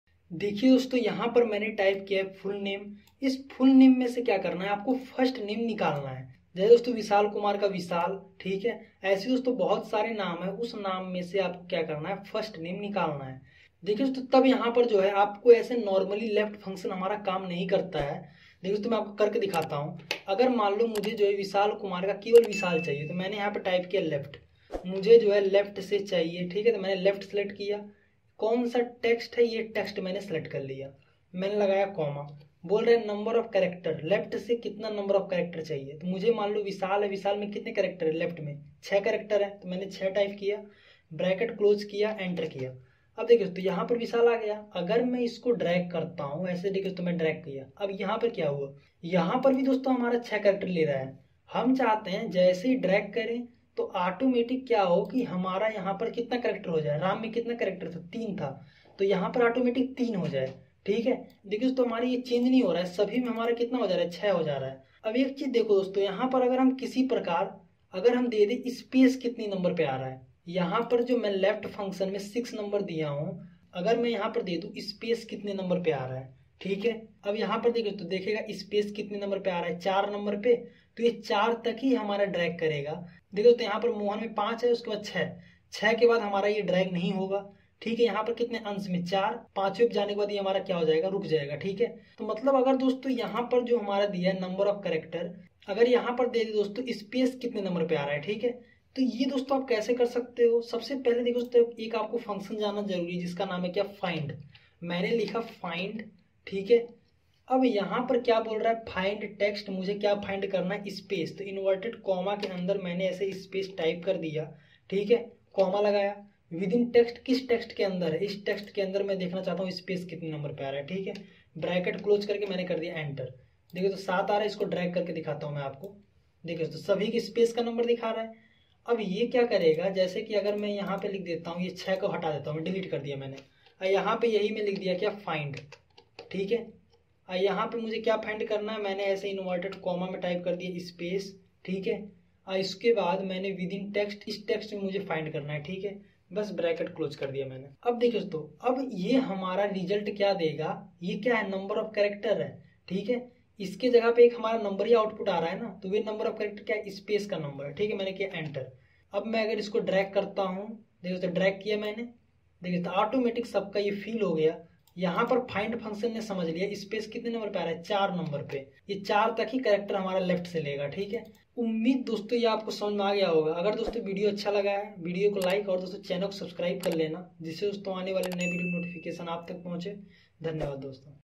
Extract First Name from Full Name | Left function formula in excel | Learnho
Get first name from full name using nested left function in excel. In this video we will learn most useful function formula. Friends normally you will know that how to use left function but in this video we will learn nested left function. I hope you like this video.
Thanks and Love
Learnho